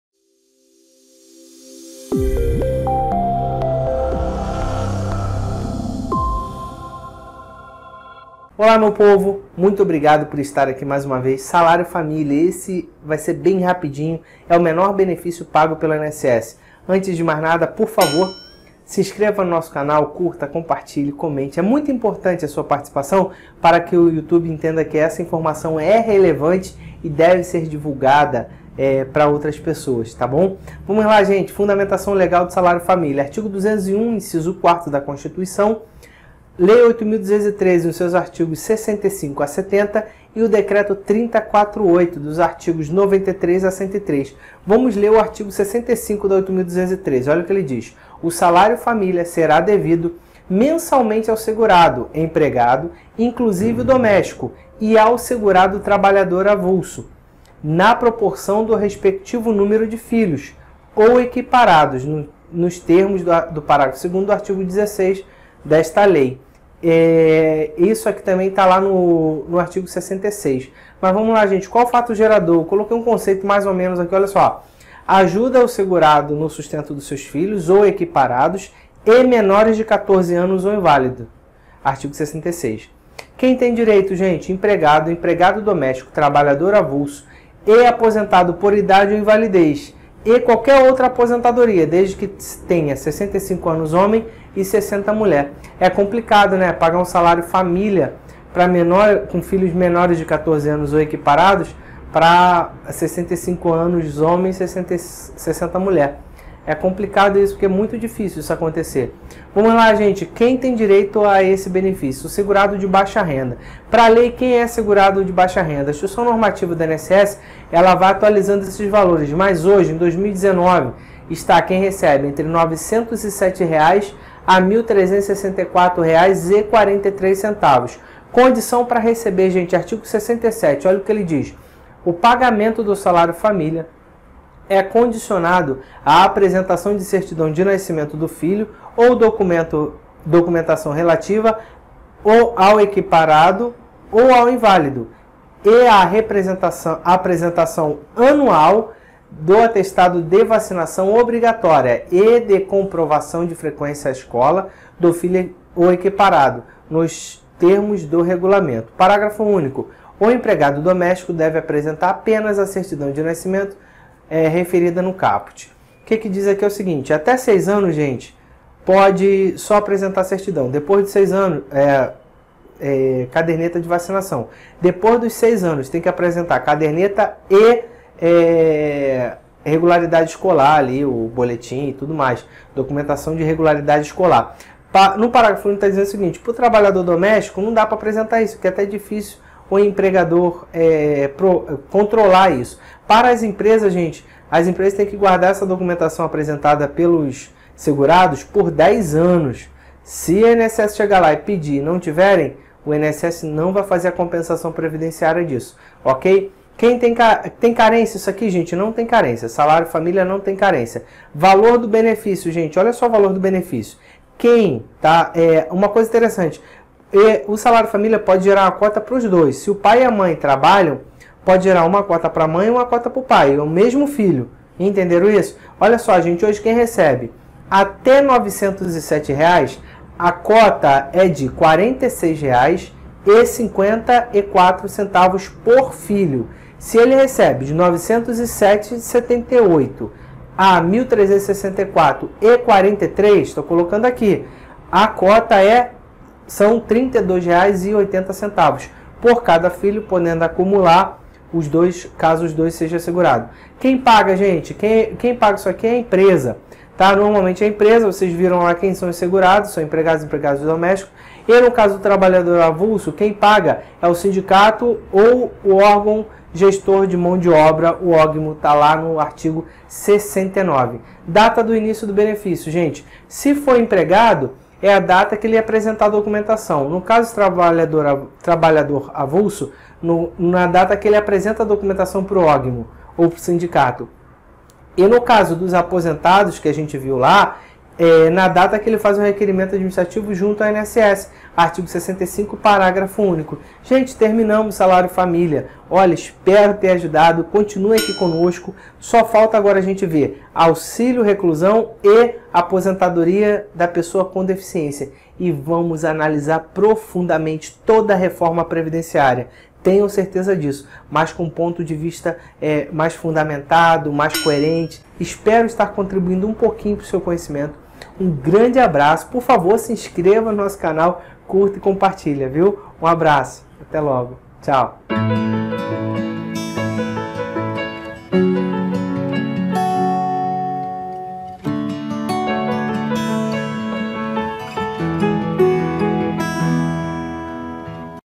SALÁRIO-FAMÍLIA - COMPLETO . REFORMA DA PREVIDÊNCIA 2019
SALÁRIO FAMÍLIA

1- Fundamentação legal: art.201, IV da CF e Lei 8.213/91. Art. 65 a 70 e Decreto 3048/99, art. 93 a 103.
Art. 65.  O salário-família será devido, mensalmente, ao segurado empregado, inclusive o doméstico, e ao segurado trabalhador avulso, na proporção do respectivo número de filhos ou equiparados  nos  termos  do  § 2o do art. 16 desta Lei, observado o disposto no art. 66.

2- FATO GERADOR: Ajudar o Segurado no sustento dos filhos ou equiparados e menores de 14 anos ou inválidos, Art.66.

3- TEM DIREITO.
*Empregado
*Empregado doméstico
*Trabalhador avulso
* e Aposentados, por idade, invalidez e qualquer outro desde que tenha mais de 65H e 60M.

Segurado de BAIXA RENDA
Quem recebe até R$ 907 até R$ 1.364,43.

4- CONDIÇÃO PARA RECEBER
Art. 67. O pagamento do salário-família é condicionado à apresentação da certidão de nascimento do filho ou da documentação relativa ao equiparado ou ao inválido, e à apresentação anual de atestado de vacinação obrigatória e de comprovação de frequência à escola do filho ou equiparado, nos termos do regulamento.
Parágrafo único.  O empregado doméstico deve apresentar apenas a certidão de nascimento referida no caput.
*Até 6 anos vacinação e depois vacinação e escola.

5- Carência: NÃO TEM CARÊNCIA

6- Valor do Benefício: É PAGO POR COTAS QUE CADA FILHO PODE GERAR COTA PARA O PAI E PARA A MÂE.
Quem recebe até R$ 907,77 terá direito a cota de R$ 46,54para cada filho;
Quem recebe entre R$ 907,78 e R$ 1.364,43 o valor da cota é de R$ 32,80 por cada filho.

PAGO PELA EMPRESA:
Art. 68.  As cotas do salário-família serão pagas pela empresa ou pelo empregador doméstico, mensalmente, junto com o salário, efetivando-se a compensação quando do recolhimento das contribuições, conforme dispuser o Regulamento.
O trabalhador avulso, vai receber pelo sindicato/OGMO. Art.69.

7- D.I.B.
Empregado é data que apresenta a documentação ao empregador, empregado doméstico.
E trabalhador avulso, é data que apresenta a documentação ao sindicato/OGMO.
E da data do requerimento ao INSS no caso do aposentado, art.65, parágrafo único.